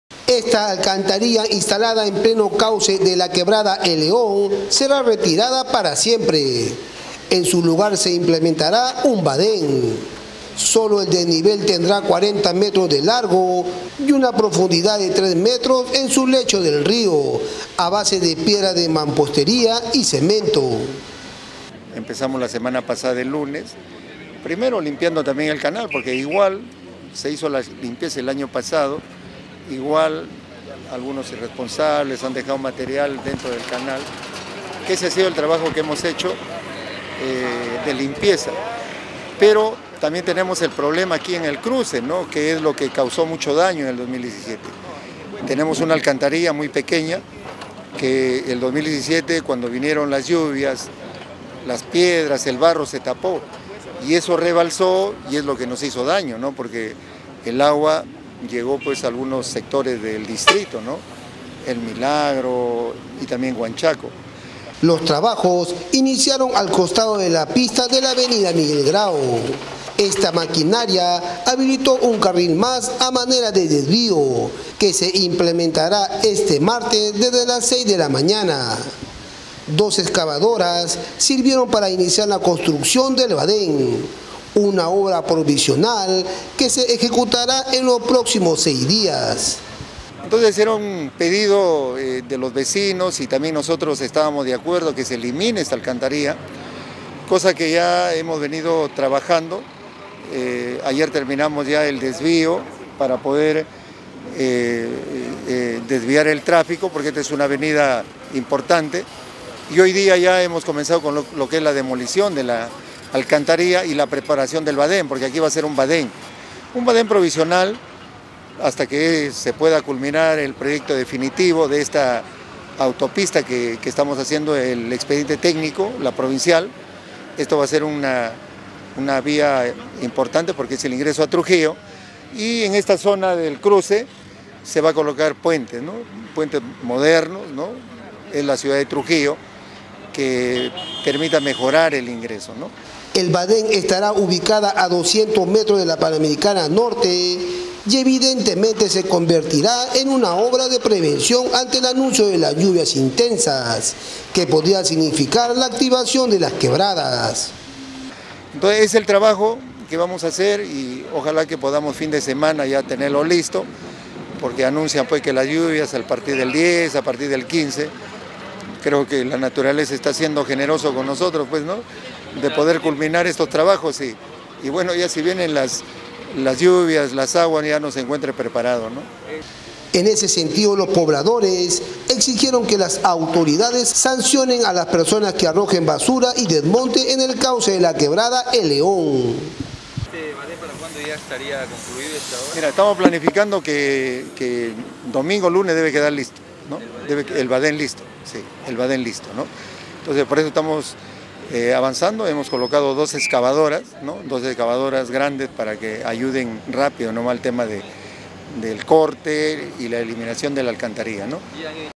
NOTA INICIAN CONSTRUCCIÓN DE BADÉN EN EL MILAGRO
Ante una posible activación de la quebrada el León Dormido, la municipalidad de Trujillo inició la construcción del badén provisional en el centro poblado El Milagro.